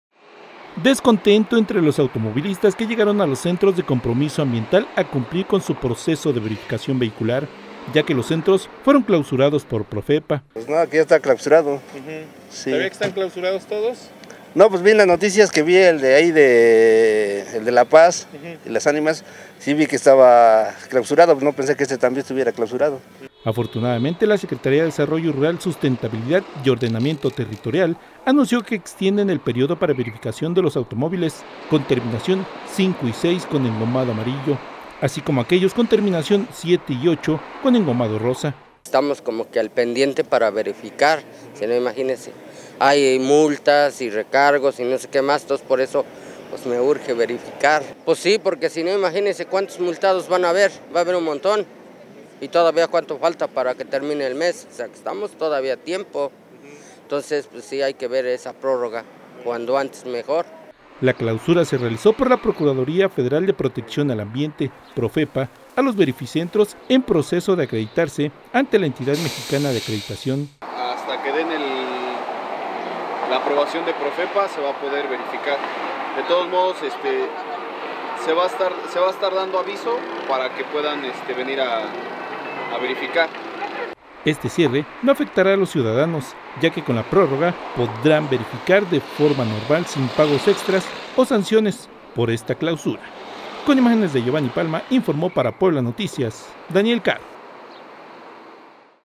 PROFEPA clausura Centros de Verificación Vehicular en Puebla
Este cierre no afectará a los ciudadanos ya que con la prórroga podrán verificar de forma normal sin pagos extras o sanciones por esta clausura.